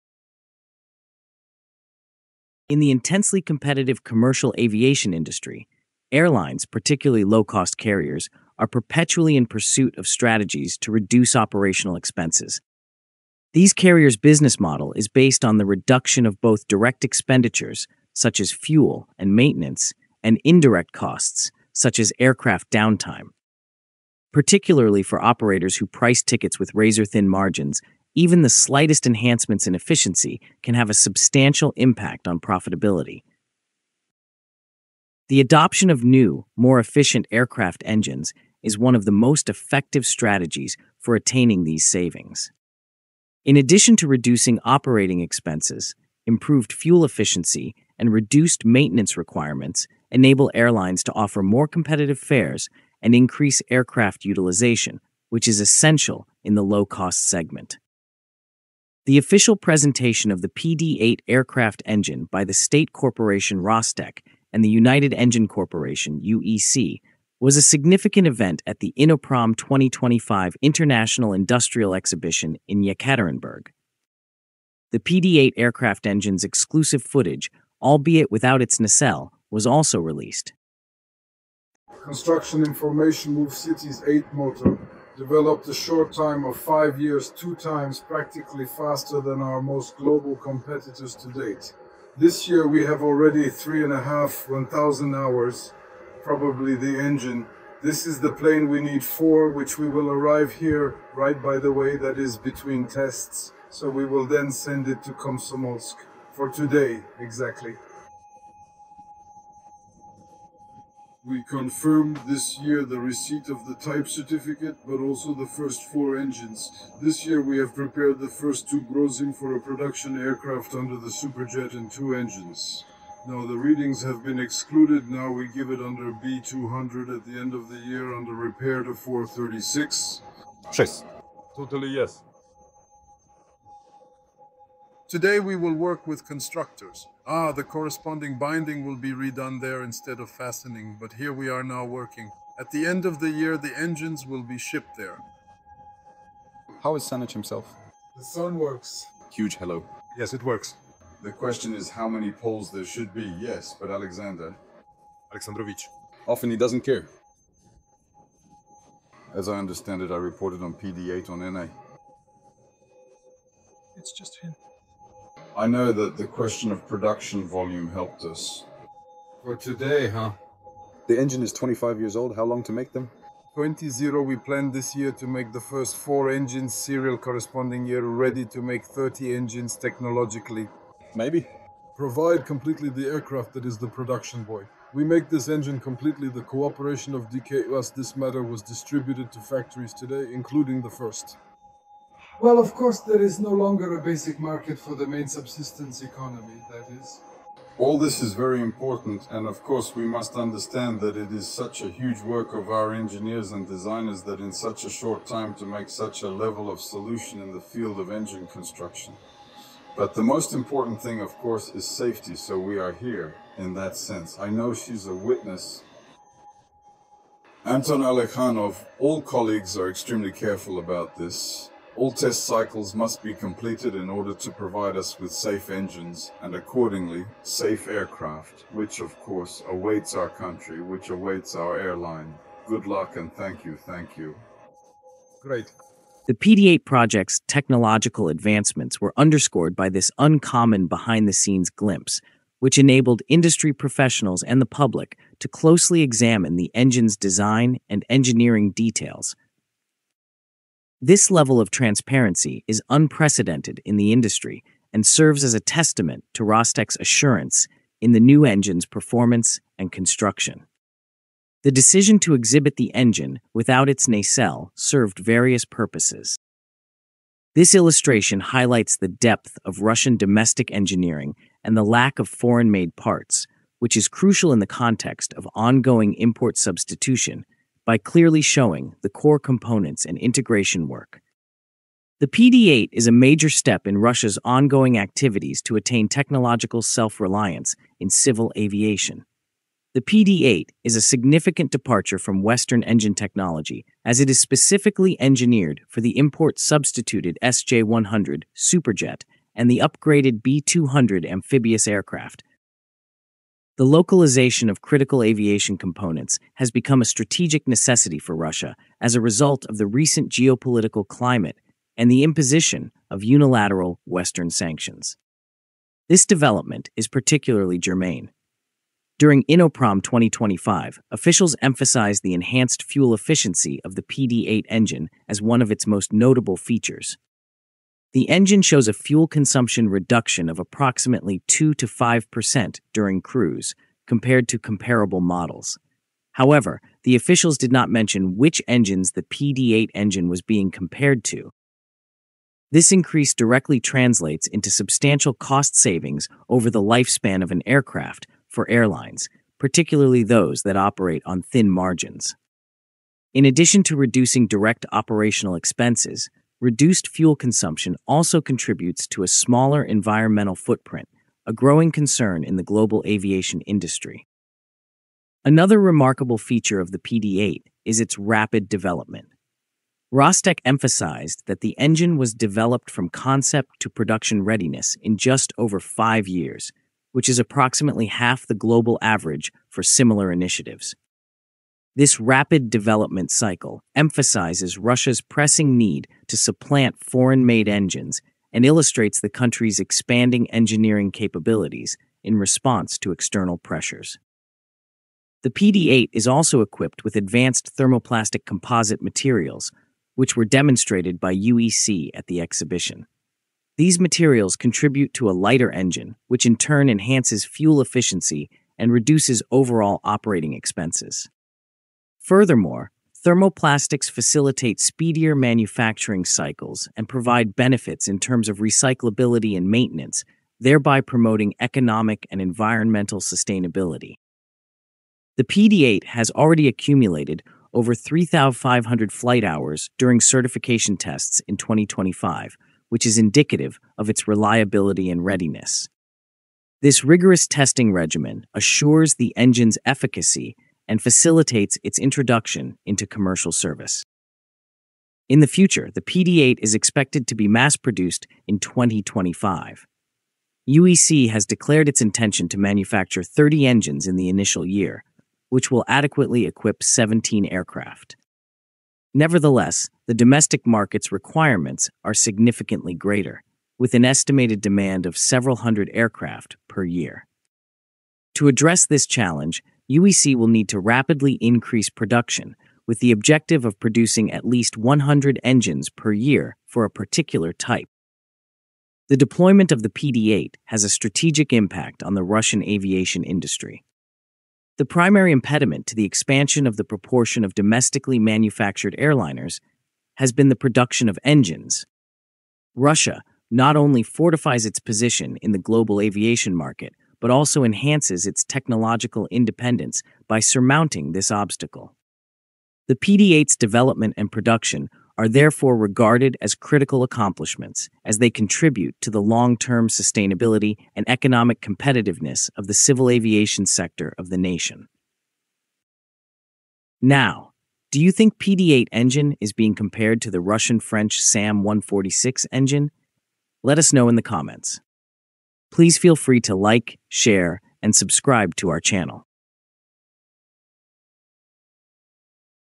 Russia’s PD-8 Engine Promises 2–5% More Cruise Efficiency
Russia’s aviation industry is making headlines with the new PD-8 engine, designed for the SJ-100 Superjet and Be-200 aircraft. Revealed at Innoprom-2025, the PD-8 promises 2–5% better fuel efficiency during cruise compared to similar engines — a huge win for airlines operating on tight margins.

In this video, we dive into how the PD-8 achieves these fuel savings, explore its advanced thermoplastic composites, and reveal why Russia built it in just five years. Plus, we discuss the engine’s role in helping Russia achieve technological independence amid global sanctions.